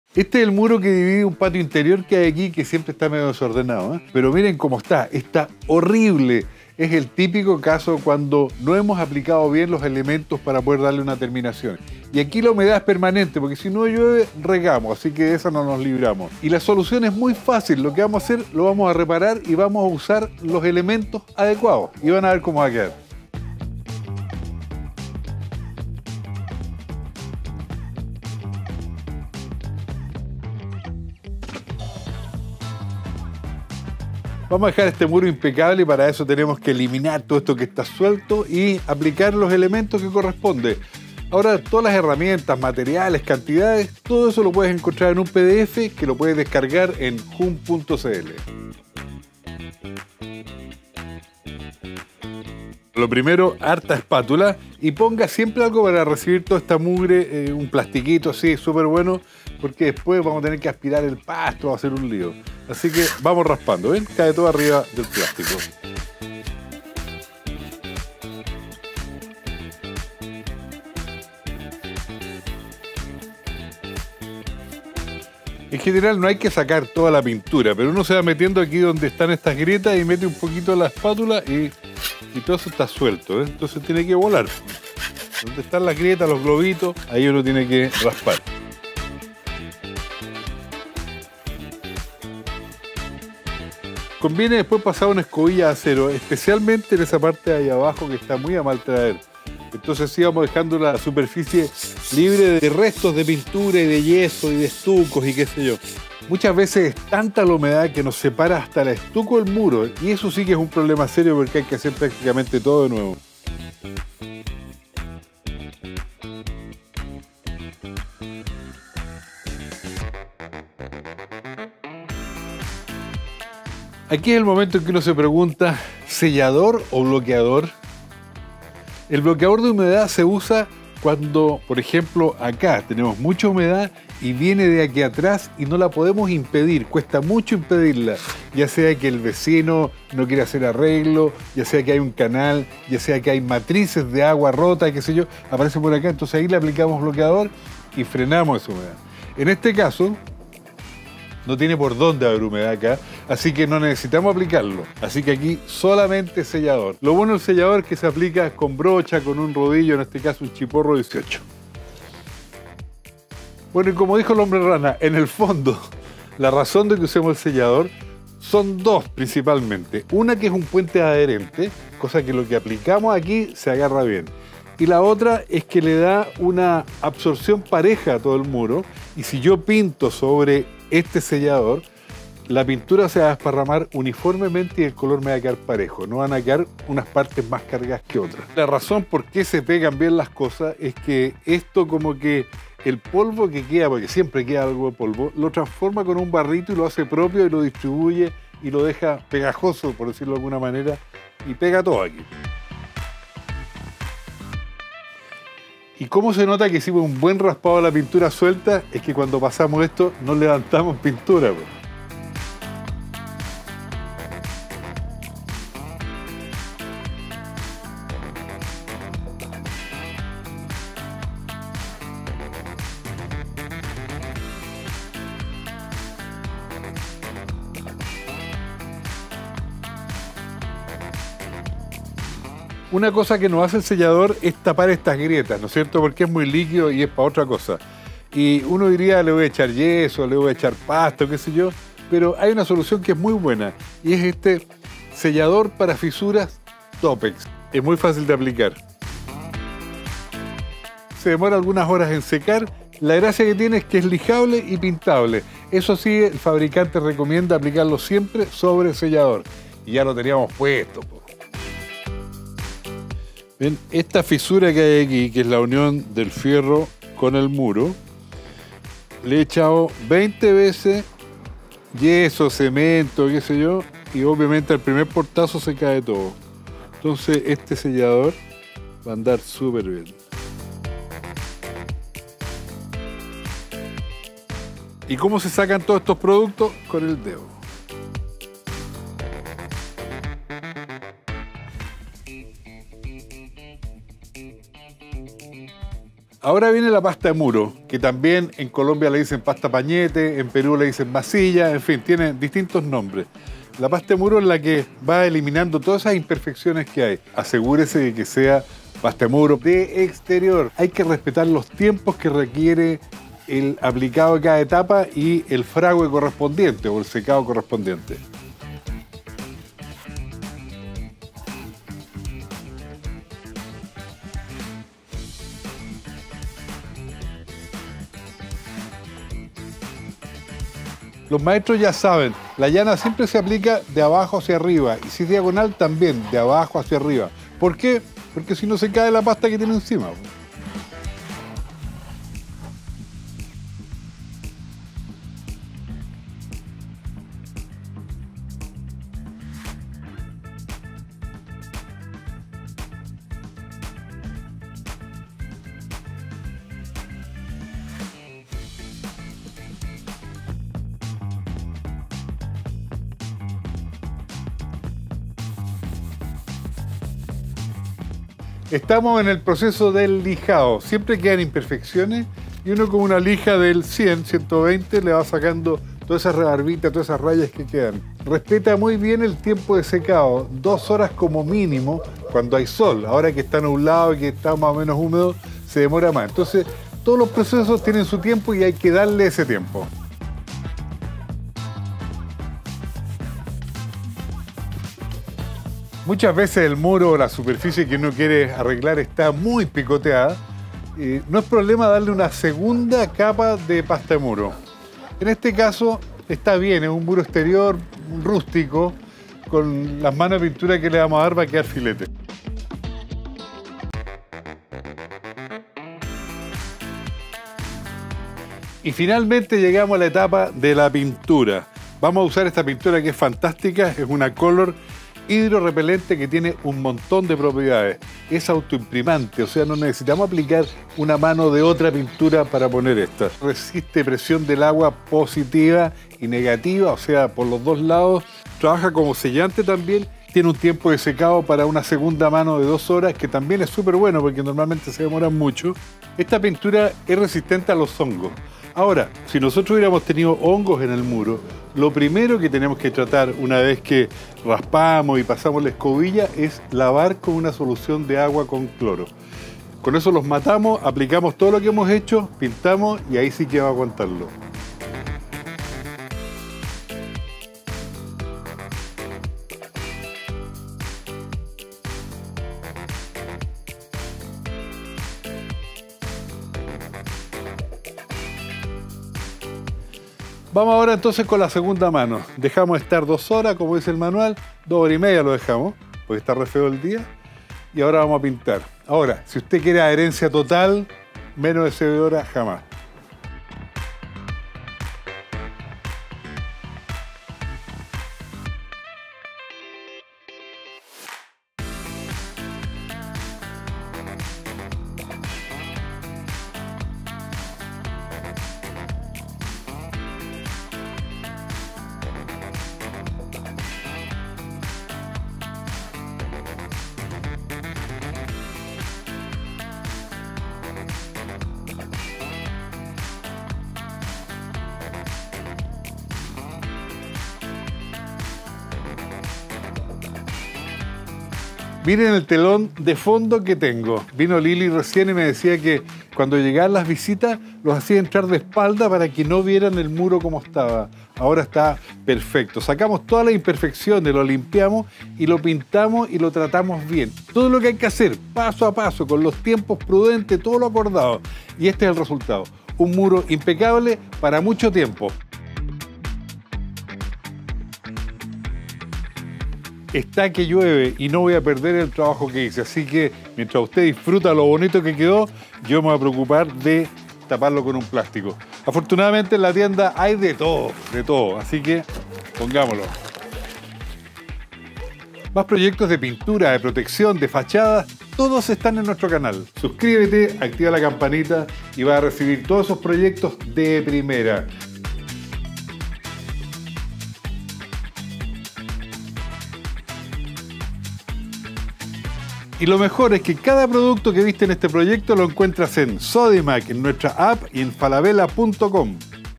Cómo quitar humedad en pared exterior
🏠🌧️ Transforma tu muro afectado por la humedad en una obra impecable. Descubre cómo eliminar la humedad en una pared exterior en este video, donde te revelamos los secretos y todo el paso a paso para lograr un resultado sorprendente. ¡Hágalo Usted Mismo!